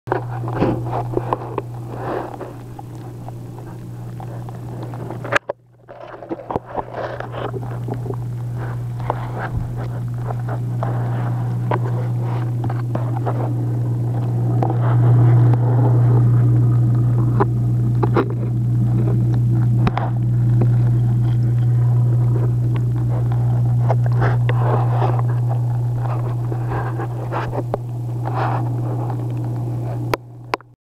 planted tank
I put my water proof camera Pentax Wg-1 into my long 20 gallon tank. As you can see this is a south american aquarium only south american fish in it.